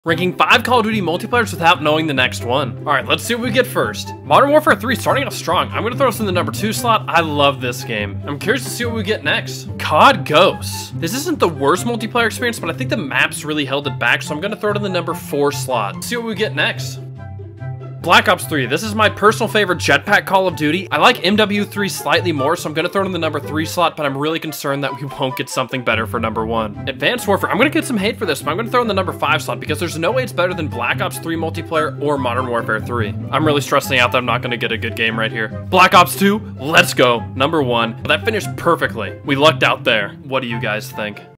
RANKING 5 COD Multiplayers Without Knowing the Next One
Ranking 5 COD Multiplayers Without Knowing the Next One! This is a COD tier list that only takes into account multiplayer! Hopefully, I can rank the CODs on this tier list correctly even without knowing what game is next. Let me know if you agree with the ranking! I love making videos for you all!